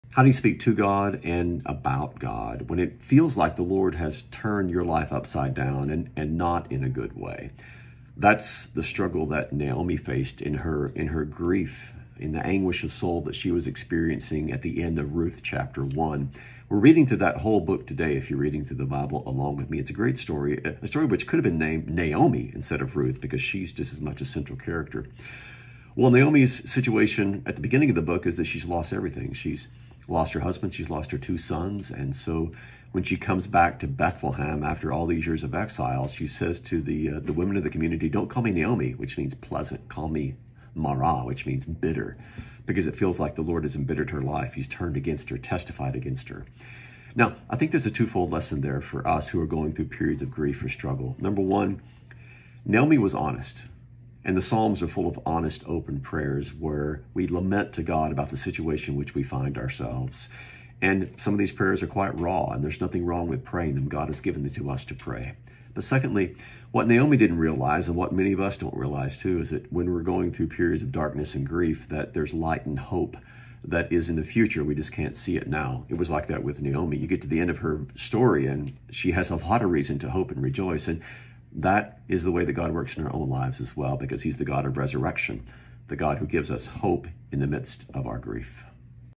What bitter Naomi teaches us about prayer and hope
When Naomi returned to Bethlehem, bereaved of her husband and two sons, she embittered and full of grief. How did she speak of God? And what did she not know, at the time, was in her future? The twofold lesson we can learn from this.